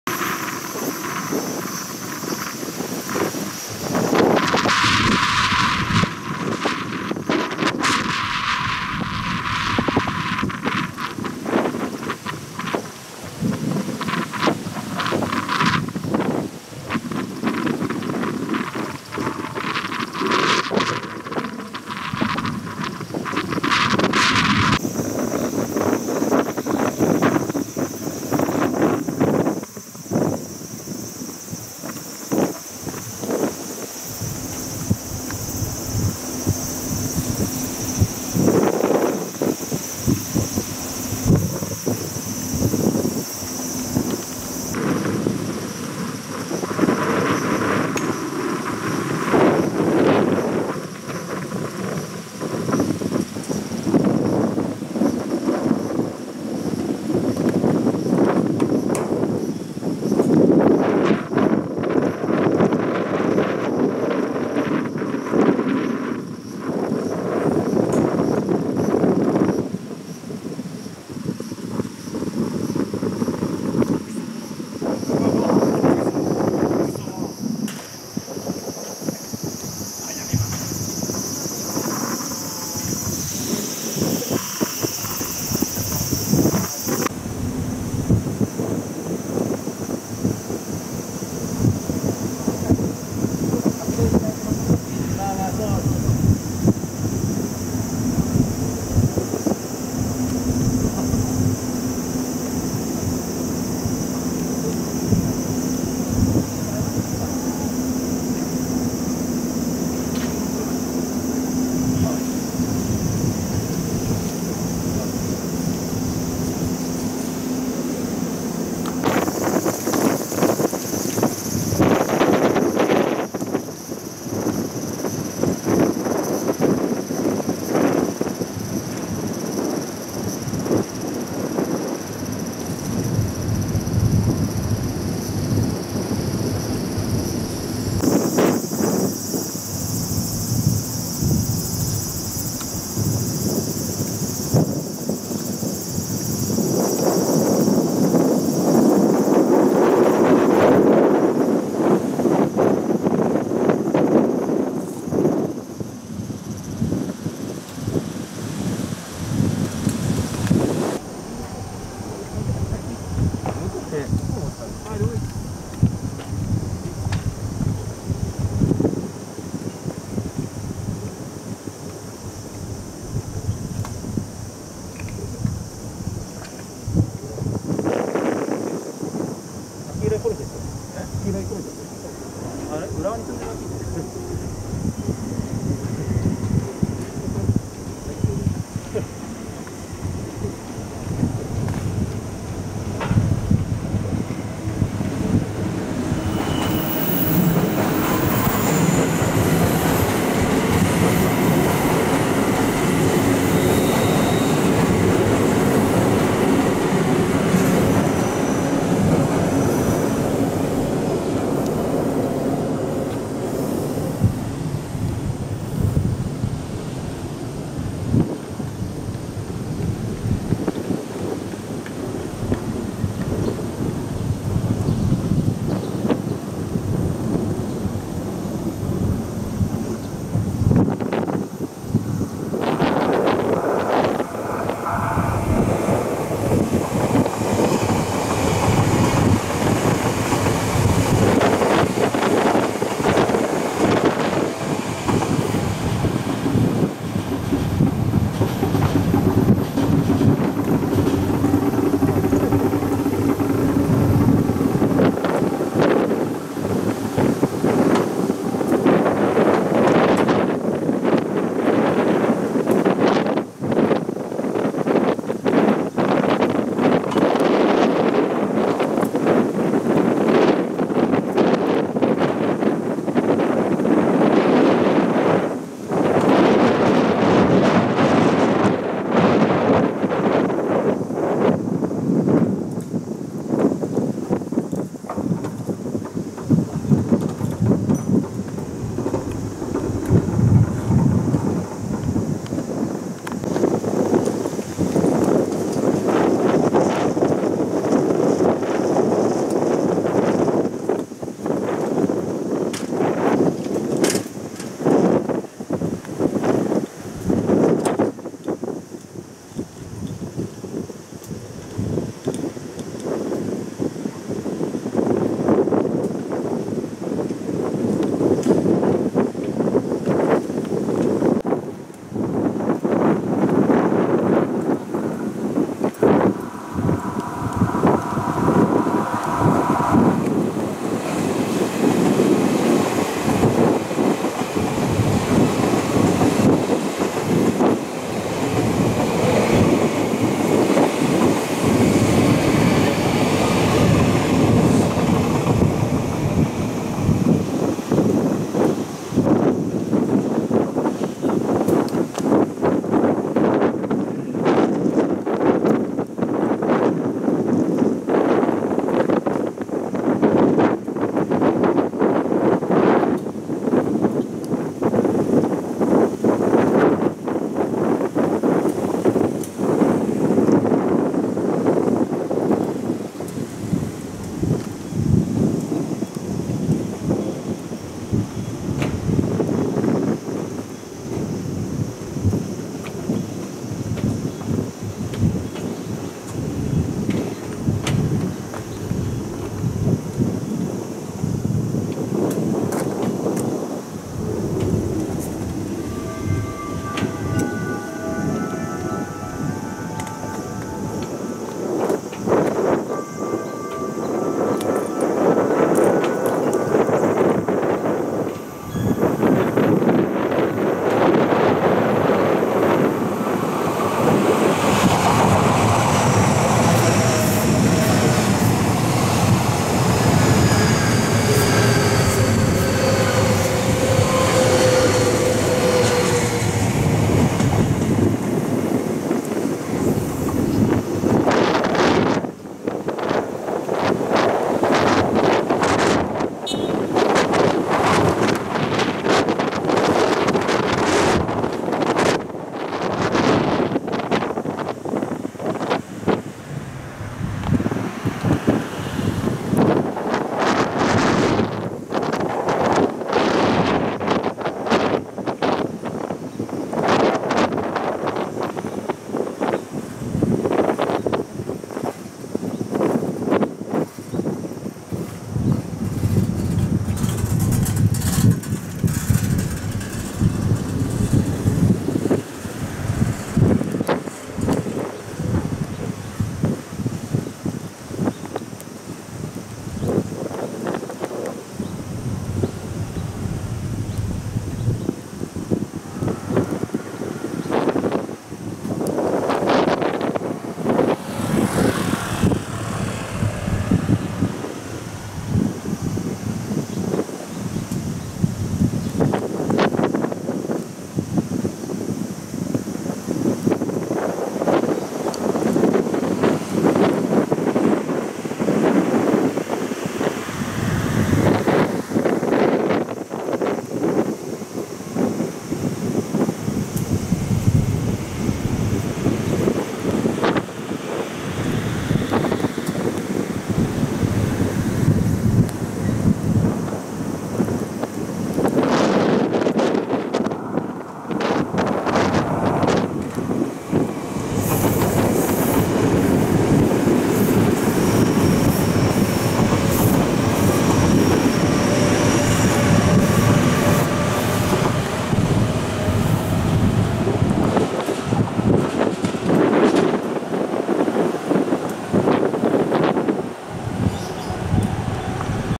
小田急 3000形SE車 大野区構内での入換･洗浄の様子 大野総合車両所 2020.09.07
2020年9月7日、大野総合車両所の車庫から初代ロマンスカーの3000形SE車が久しぶりに顔を出し、洗浄線にて洗浄が行われた後に構内で入換作業が行われました。
LSEと同様に、近い内に海老名のロマンスカーミュージアムへ輸送されると思います。